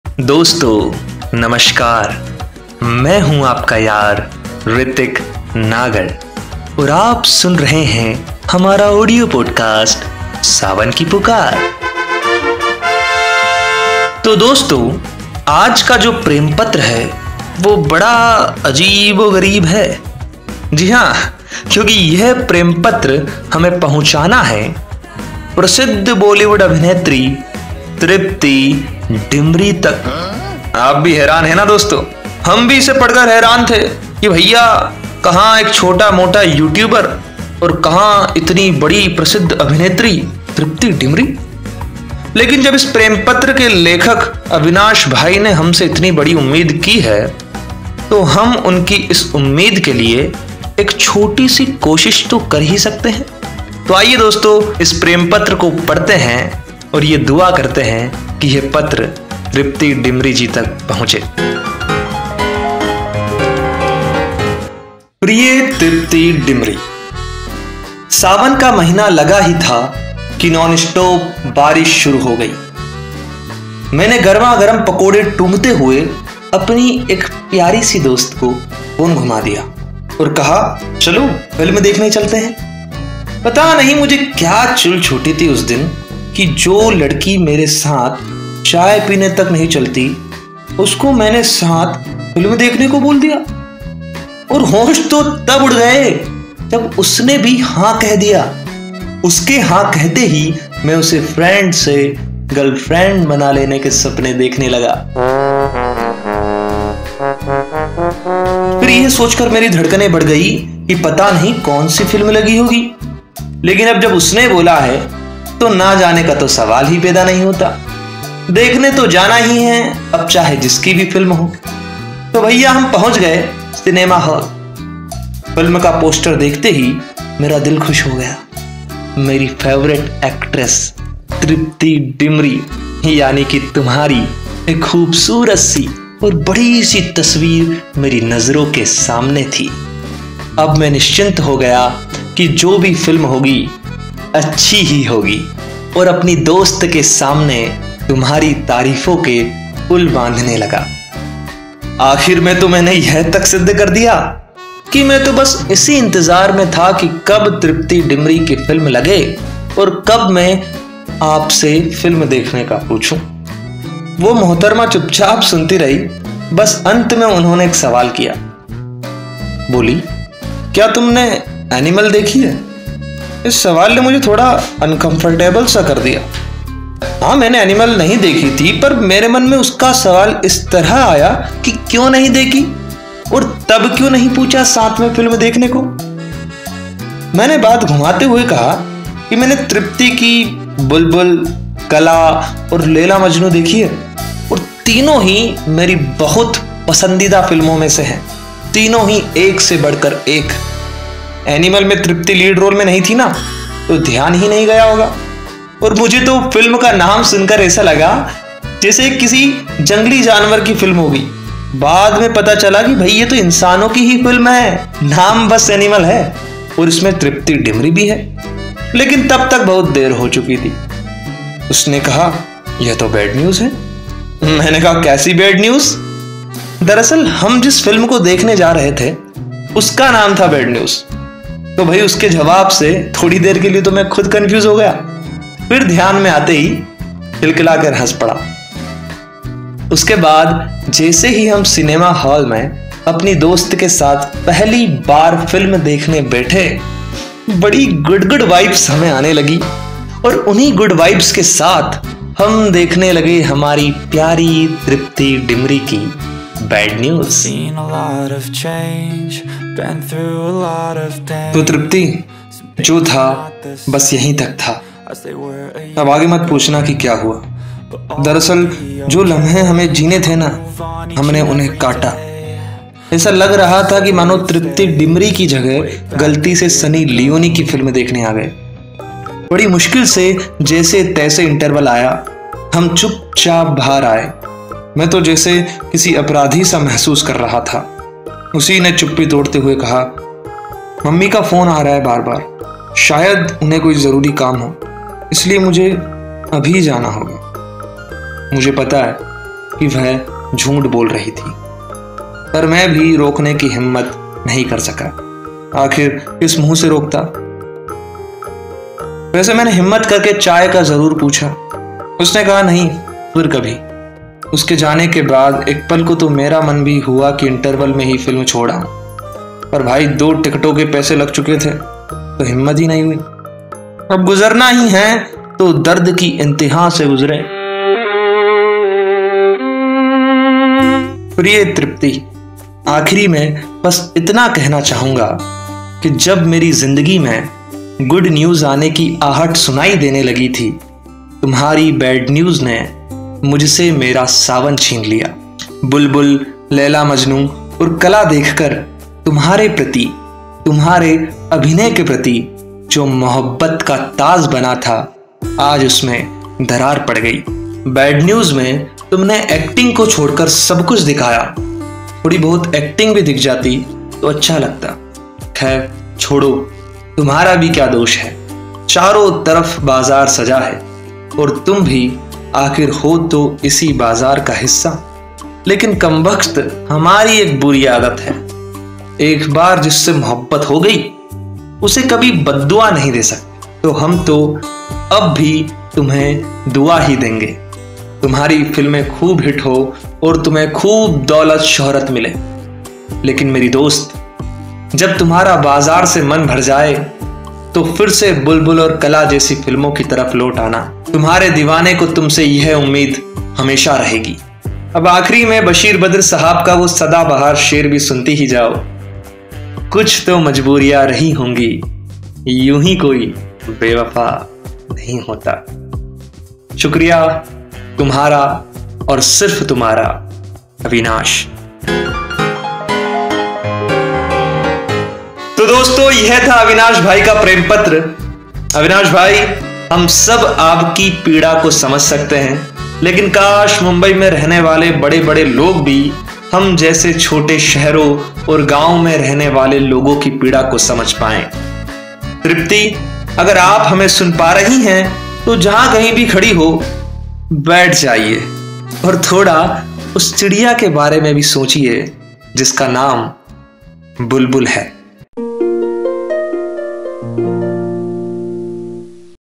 Tripti Dimri | A Love Letter to Bulbul, Qala and Laila-Majnu Fame Tripti Dimri
मेरी दोस्त जब तुम्हारा बाज़ार से मन भर जाए तो फिर से बुलबुल और कला जैसी फिल्मों की तरफ़ लौट आना। तुम्हारे दीवाने को तुमसे यह उम्मीद हमेशा रहेगी।